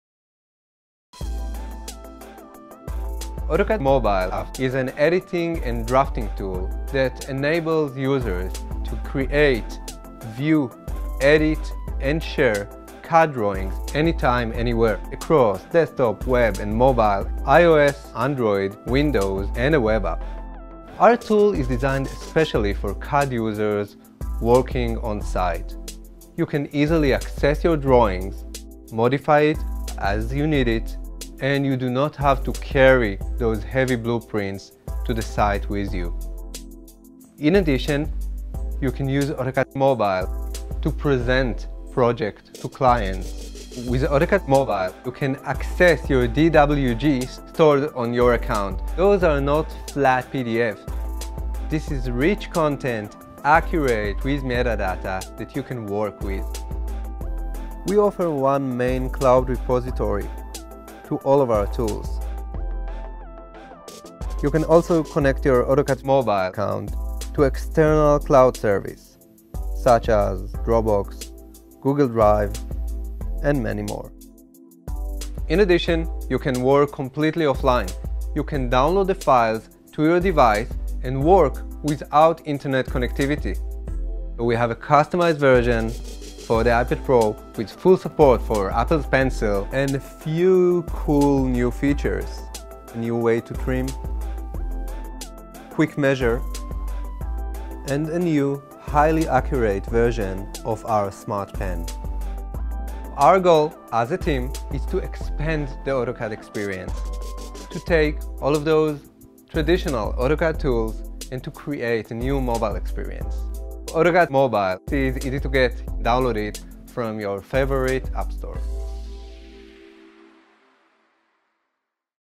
AutoCAD mobile app
View, create, edit, and share CAD drawings on your mobile device with the AutoCAD mobile app.